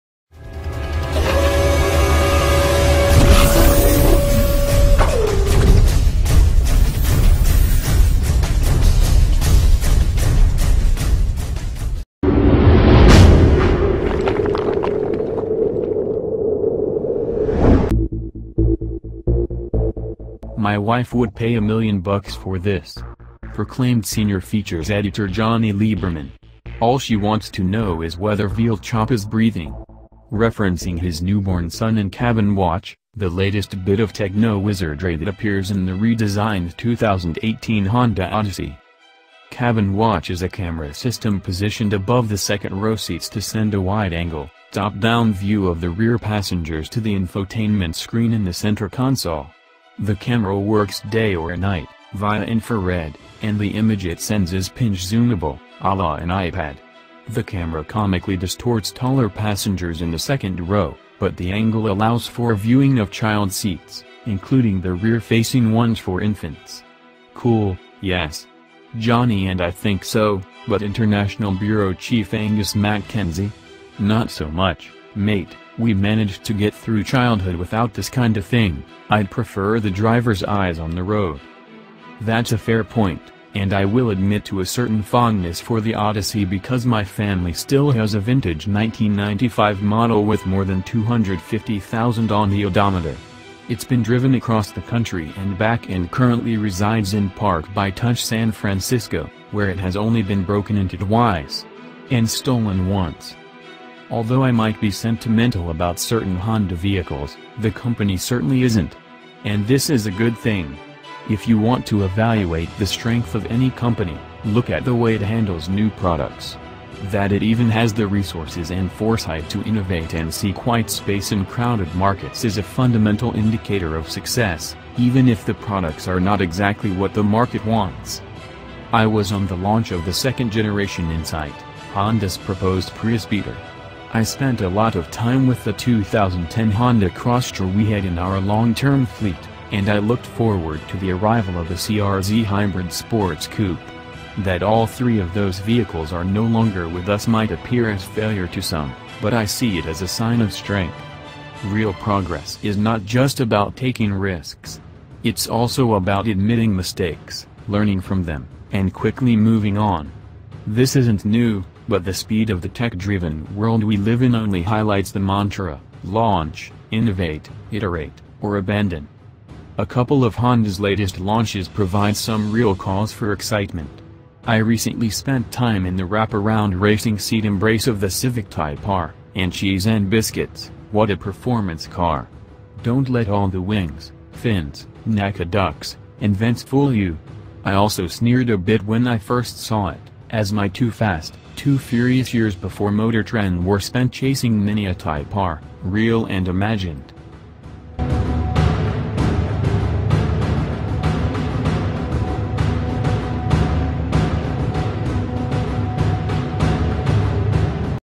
FROM CROSSTOUR TO NEW HONDA CIVIC HYBRID TYPE R: HOW HONDA STAYS STRONG – THE LOHDOWN
new honda civic hybrid, My spouse would pay 1,000,000 greenbacks for this! announced senior options editor Jonny Lieberman. All she desires to understand is whether or not veau Chop is breathing! referencing his newborn son and Cabin Watch, the most recent little bit of techno creative thinking that seems within the redesigned 2018 Honda Odyssey.

Cabin Watch may be a camera system positioned higher than the second-row seats to send a optical lens, top-down read of the rear passengers to the documentary film screen within the center console. The camera works day or night (via infrared), and also the image it sends is pinch-zoomable, à la associate degree iPad. The camera funny distorts taller passengers within the second row, however the angle permits for viewing of kid seats, together with the rear-facing ones for infants. Cool, yes? Jonny and that i assume thus, however international bureau chief Angus macintosh Kenzie? Not thus much: Mate, we tend to managed to urge through childhood while not this rather thing; I’d like the driver’s eyes on the road.

That’s a good purpose, and that i can admit to an explicit fondness for the Odyssey as a result of my family still incorporates a vintage 1995 model with over 250,000 on the milometer. It’s been driven across the country and back and presently resides in park-by-touch city, wherever it's solely been broken into doubly. And purloined once.

Although i would be sentimental concerning bound Honda vehicles, the corporate definitely isn’t. And this is often an honest factor. That it even has the resources and foresight to pioneer and request white house in huddled markets may be a elementary indicator of success—even if the merchandise don't seem to be precisely what the market desires.

I was on the launch of the second-generation Insight, Honda’s projected Prius-beater. I spent plenty of your time with the 2010 Honda Crosstour we tend to had in our long-run fleet, and that i looked forward to the arrival of the CR-Z hybrid sports car. that every one 3 of these vehicles are not any longer with North American nation would possibly seem as failure to some, however I see it as an indication of strength. Real progress isn't close to taking risks. It’s additionally concerning admitting mistakes, learning from them, and quickly moving on. This isn’t new, however the speed of the tech-driven world we tend to sleep in solely highlights the mantra: launch, innovate, iterate, or abandon.

A couple of Honda’s latest launches give some real cause for excitement. I recently enjoyed time within the garment sport seat embrace of the Civic kind R, and cheese-n-biscuits, what a performance car! Don’t let all the wings, fins, NACA ducts, and vents fool you. I additionally sneered a trifle after I initial saw it—as my too-fast, too-furious years before Motor Trend were spent chasing many an kind R, real and fanciful.